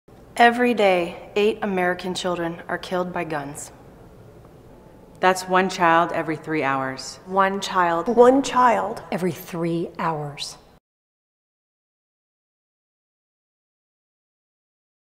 MomsDemandAction - 15 seconds
Moms Demand Action was founded to demand action NOW to:

1) Ban assault weapons and ammunition magazines that hold more than 10 rounds.
2) Require background checks for all gun and ammunition purchases.

3) Report the sale of large quantities of ammunition to the ATF, and ban online sales of ammunition.
4) Counter gun industry lobbyists' efforts to weaken gun laws at the state level.